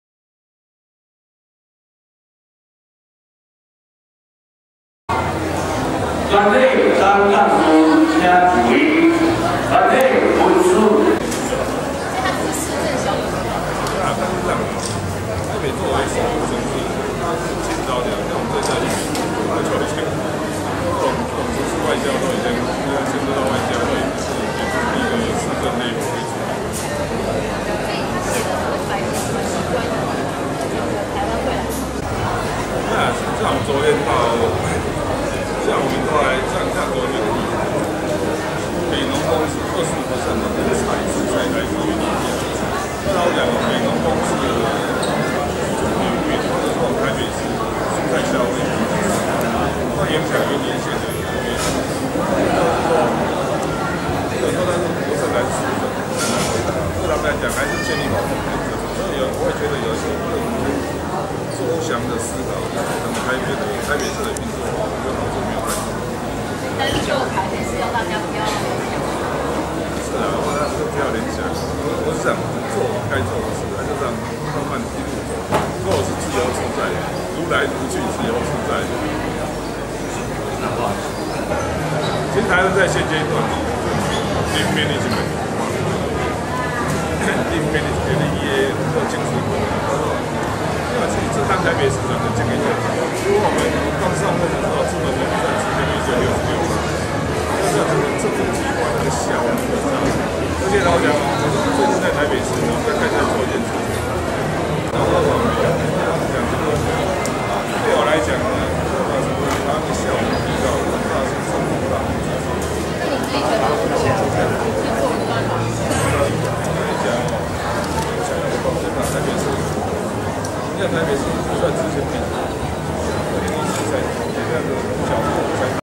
撰寫國政白皮書 柯文哲：別聯想太多
（中央社記者葉子綱雲林縣13日電）台北市長柯文哲邀專家學者撰寫國政白皮書，他今天在雲林表示，做該做的事，不要聯想太多，與外國的城市外交已不是市政層級可以處理，自由自在，如來如去。